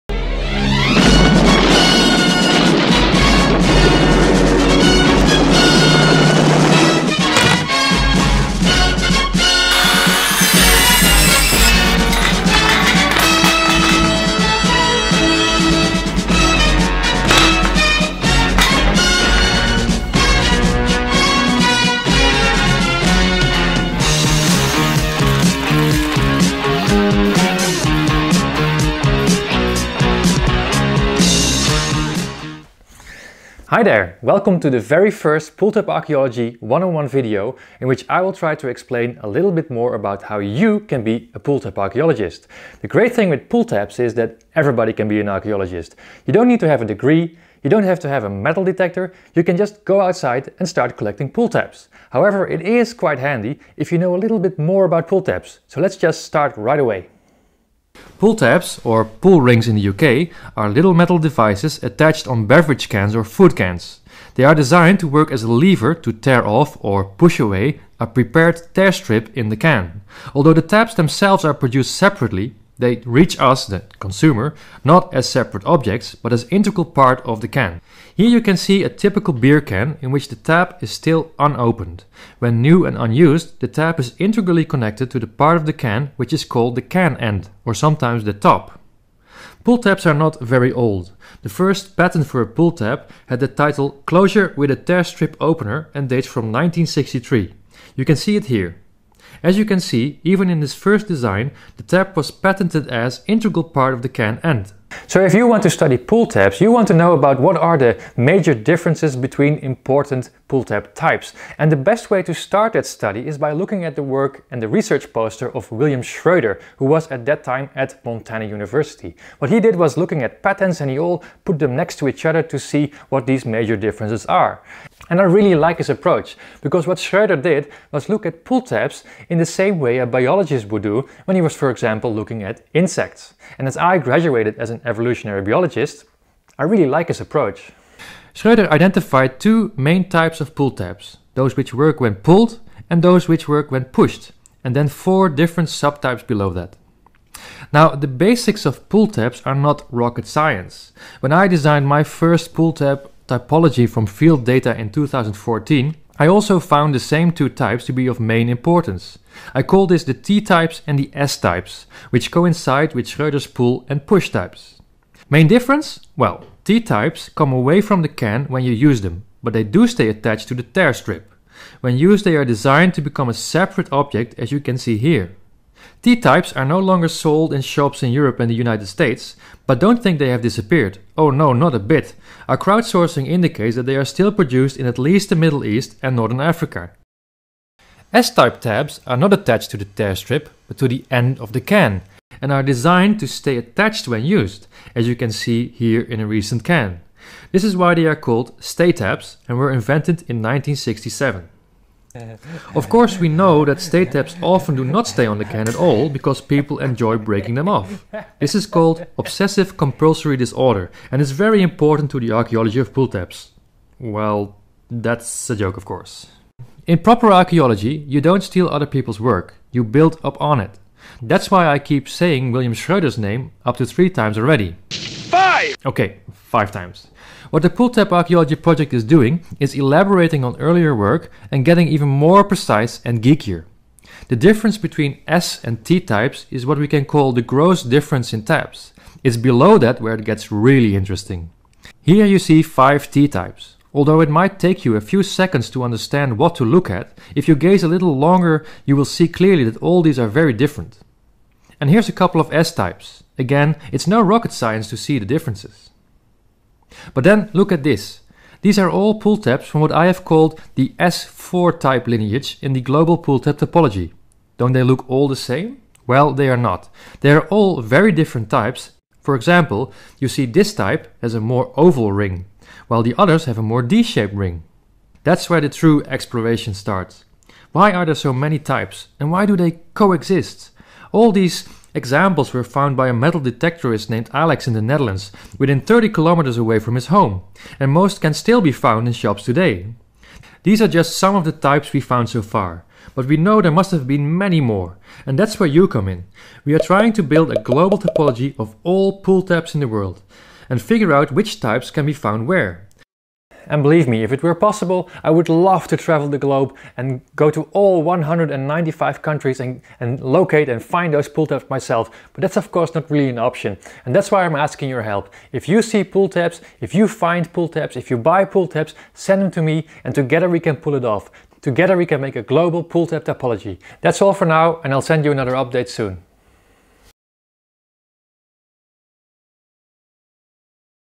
Pull Tab Archaeology 101
Finaly, some actual information about pull tabs, to help you be a pull tab archaeologist.
The main information is about T-types and S-types. Good luck and please help us make a global typology by sending in pull tabs.

Oh and yes, there is a typo in the url banner. Dyslectic. ;-)

or facebook and instagram.